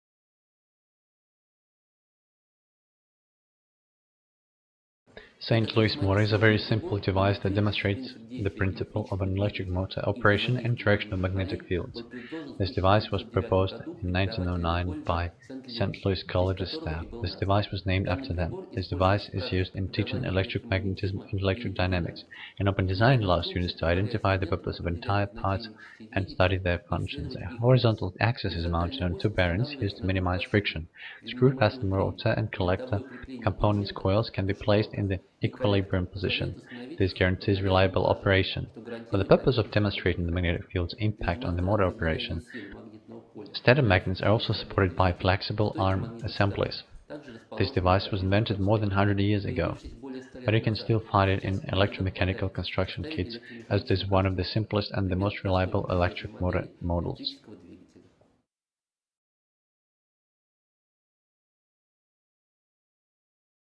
St. Louis Motor
St. Louis MotorSt. Louis motor is a very simple device that demonstrates the principle of an electric motor operation and interaction of magnetic fields.  This device was proposed in 1909 by St. Louis College's staff. The device was named after them. This device is used in teaching electric magnetism and electric dynamics.An open design allows students to identify the purpose of engine parts and study their functions.A horizontal axis is mounted on two bearings used to minimize friction. Screw-fastened rotor and collector components (coils) can be placed in the equilibrium position. This guarantees reliable operation.For the purpose of demonstrating the magnetic field's impact on the motor operation, stator magnets are also supported by flexible arm assemblies.This device was invented more than hundred years ago. But you can still find it in electromechanical construction kits as it is one of the simplest and the most reliable electric motor models.